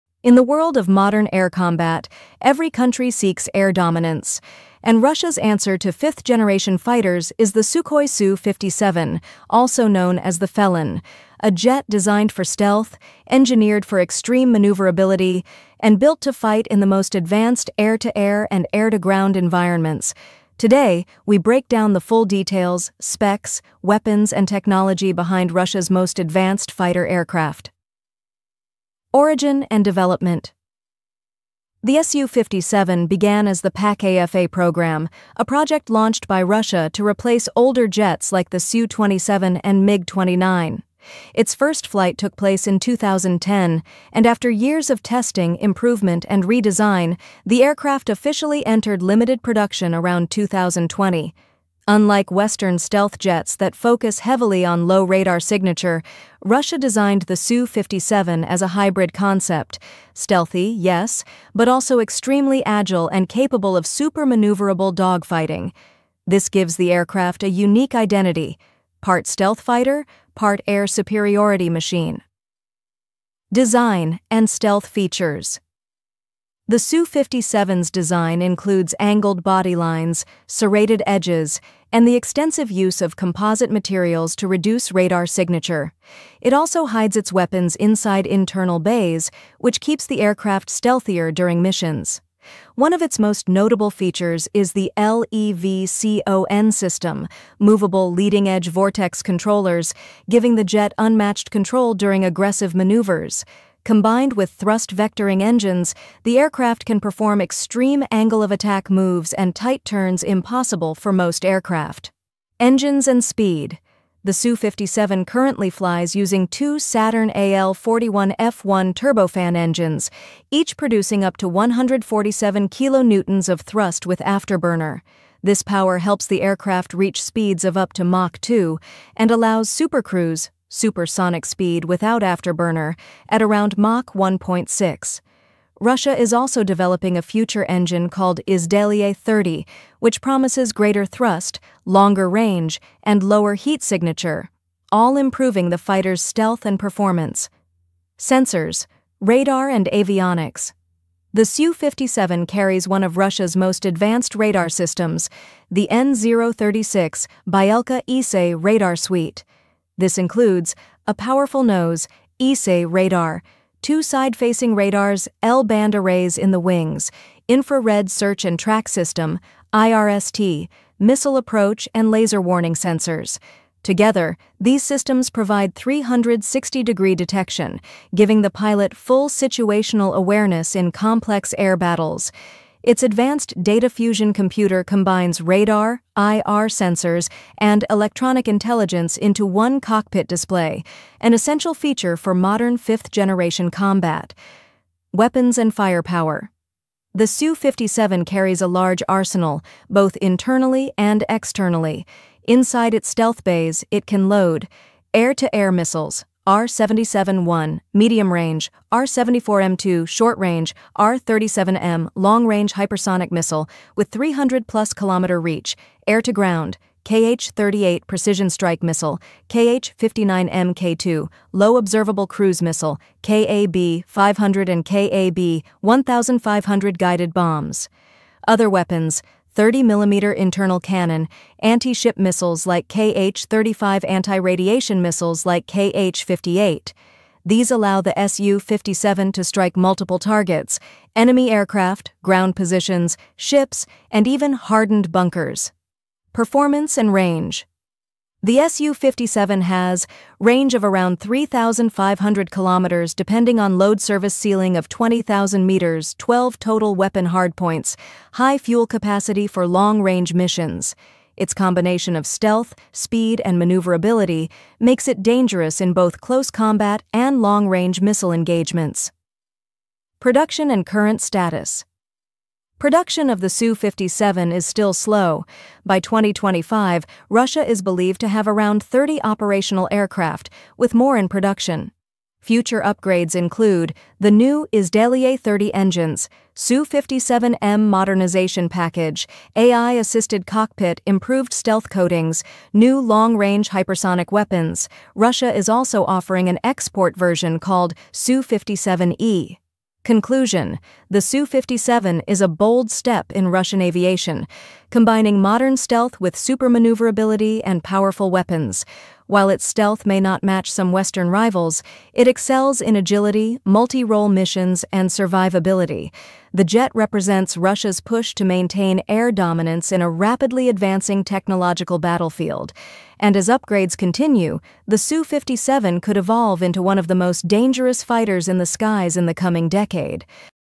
Russia’s Su-57 Fighter Jet – Full Specs, Performance & Weapons Breakdown
Russia’s Su-57 “Felon” is one of the most advanced 5th-generation fighter jets in the world.
In this video, we break down the full specs, stealth features, speed, engines, radar system, weapons, and real combat capabilities of the Su-57